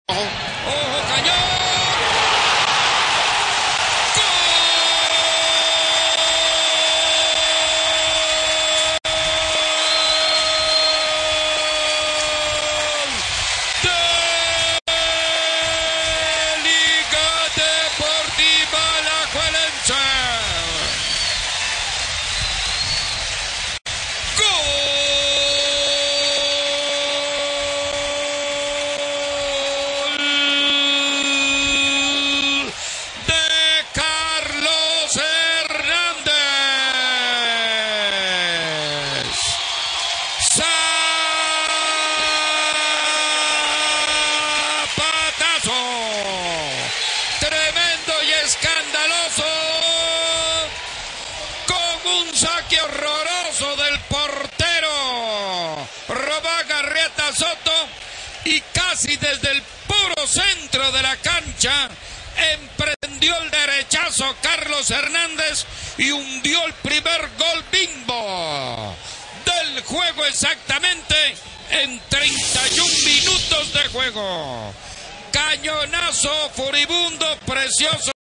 Gol de Carlos Hernandez a San Carlos
Gol de Carlos Hernandez a San Carlos, Liga 1 San Carlos 0, cuartos de final clausura 2007